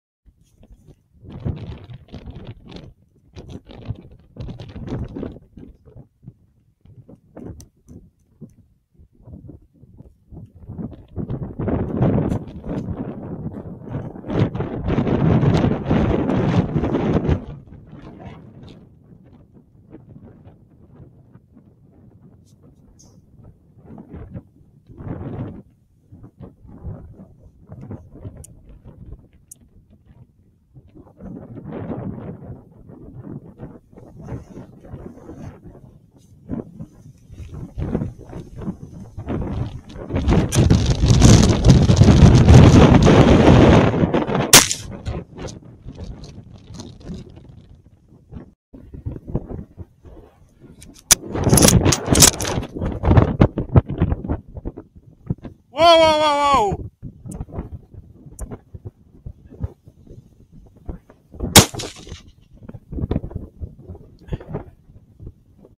windy night
Ridiculously windy in central vic tonight .
But out wandering around a small farmlet seeing as thou there was no moon and the weather was dropping / front coming in .
Seriously didn't think id see anything but the first a dog fox was happily wandering , marking his territory .
The second a vixen came charging into the Tenterfield ..
Both in really good condition and both already in winter coat ..